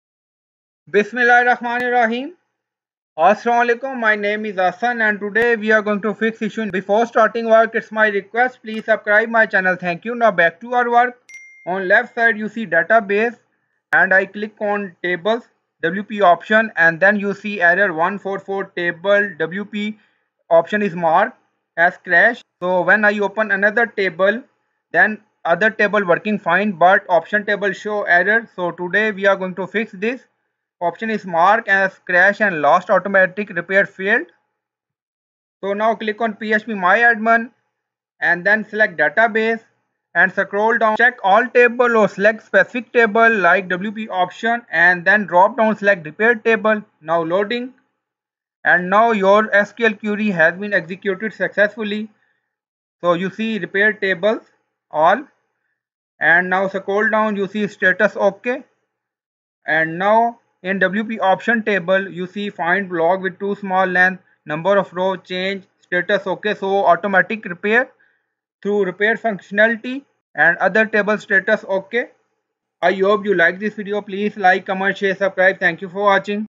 How to Fix MySQL Table is Marked as Crashed and Last (Automatic?) Repair Failed | 144 Error
In this phpmyadmin tutorial for beginners you will learn how to fix mysql error #144 - Table _options' is marked as crashed and last (automatic?) repair failed in database in phpmyadmin in localhost / cpanel.